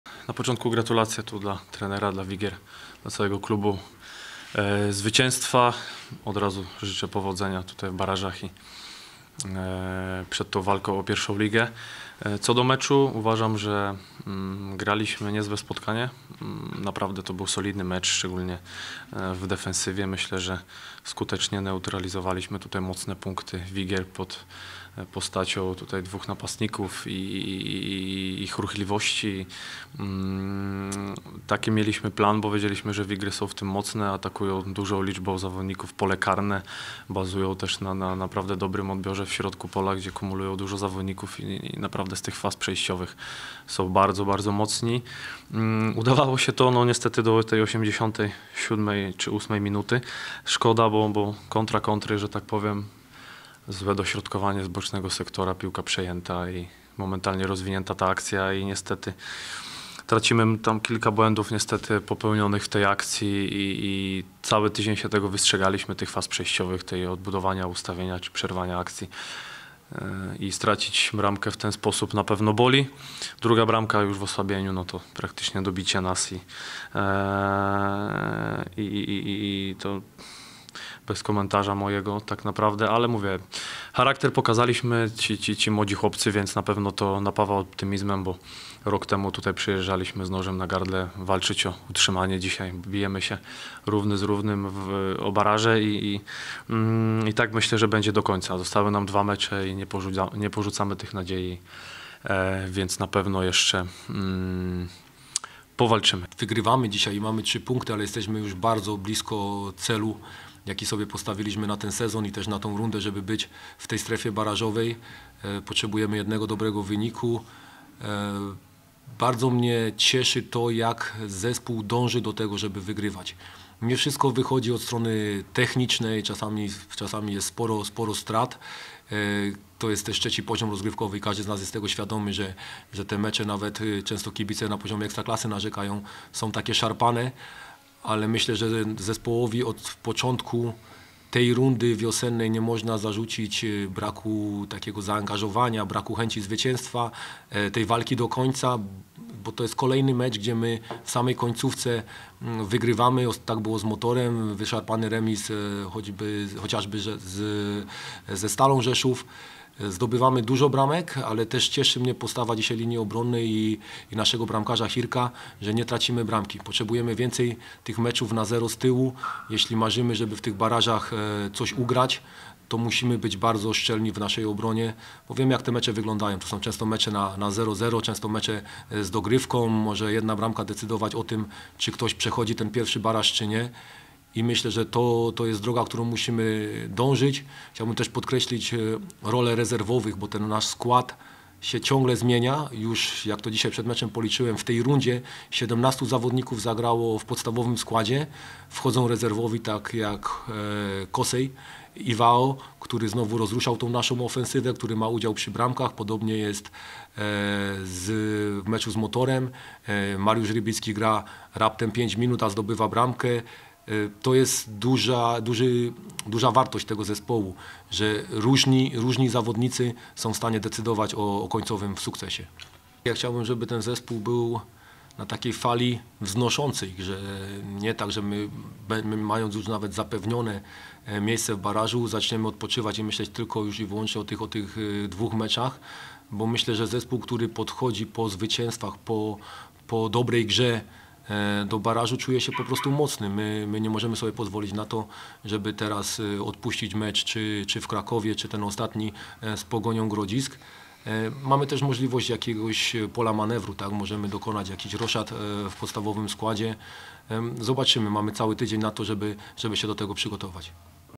KONFERENCJA | Wigry Suwałki 2:0 (0:0) Lech II Poznań
Zwycięstwo z Lechem i korzystny układ wyników dał biało-niebieskim pewny udział w barażach o awans na zaplecze Ekstraklasy. Sprawdźcie, jak wczorajszy mecz ocenili trenerzy - Artur Węska i Grzegorz Mokry.